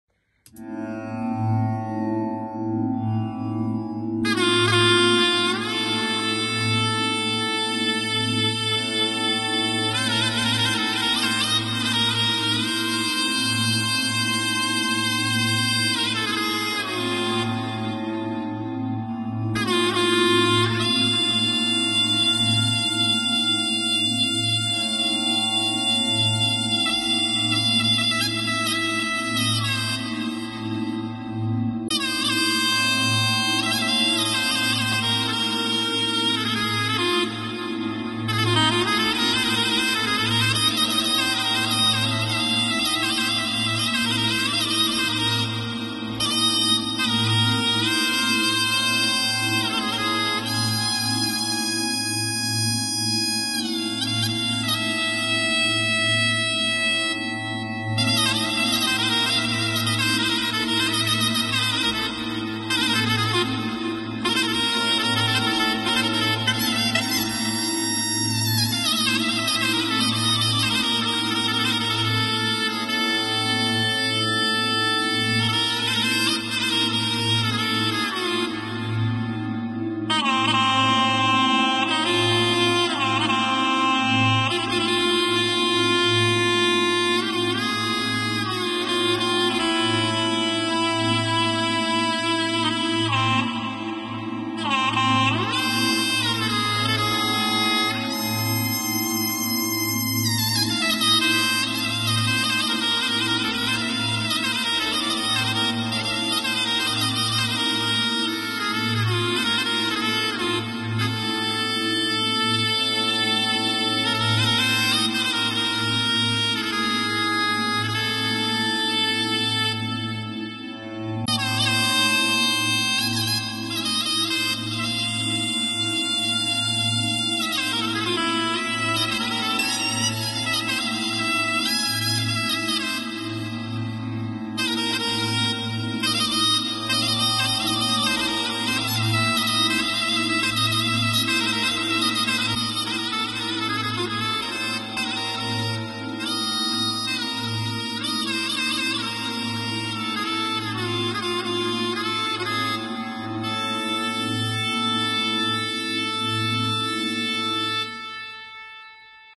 Imran Akhtar's Creative Shehnai Performance | Musical Artistry at Its Finest!
Immerse yourself in the enchanting world of music with the masterful shehnai performance by Imran Akhtar. In this captivating video, Imran showcases his creative flair, bringing a unique and artistic touch to the traditional shehnai instrument. Prepare to be spellbound by the melodic brilliance and innovation that define Imran Akhtar's musical genius. Join us on this sonic journey that transcends boundaries and celebrates the fusion of tradition and creativity.